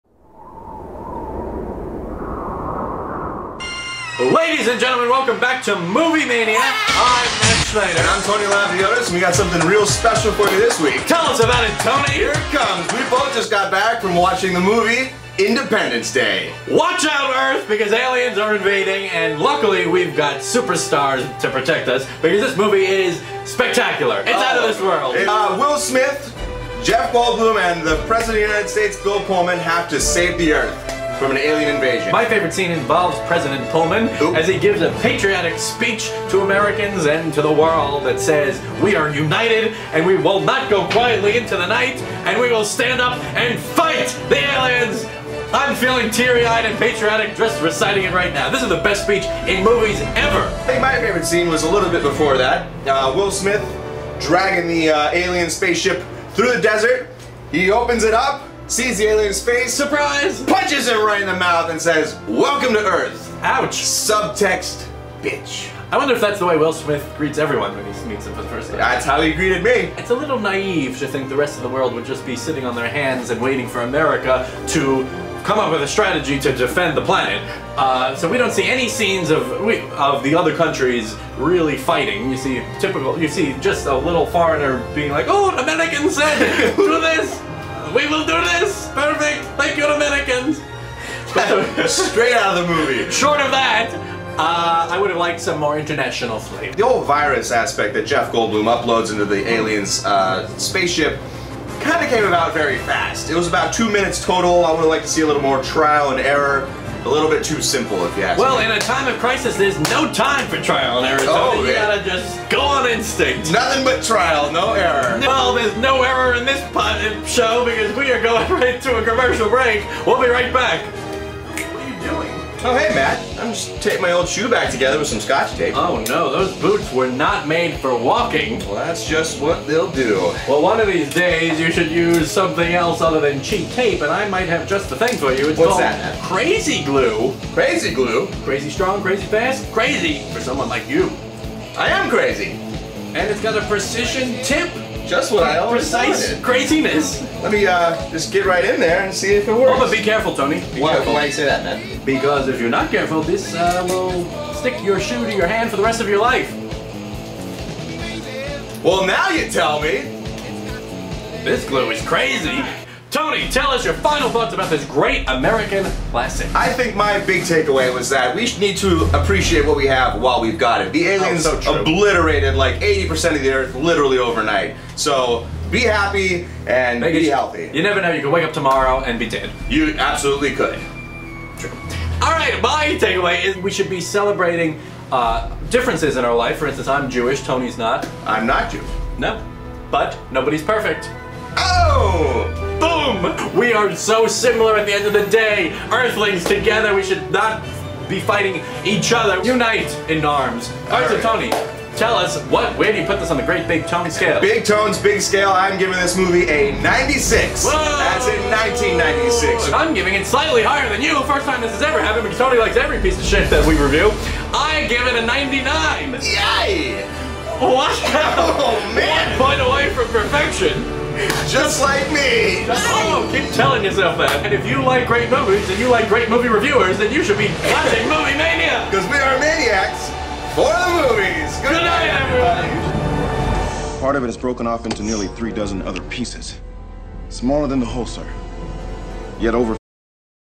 Movie Mania - Independence Day - 3 Minute Crazy Movie Review
"Movie Mania" - 3 minute movie reviews of all time favorites AT THE TIME OF THEIR RELEASE. Travel back to 1996 when this week's video was recorded as movie critics Tony Lavdiotis and Matt Schneider critique the instant American classic movie "Independence Day!"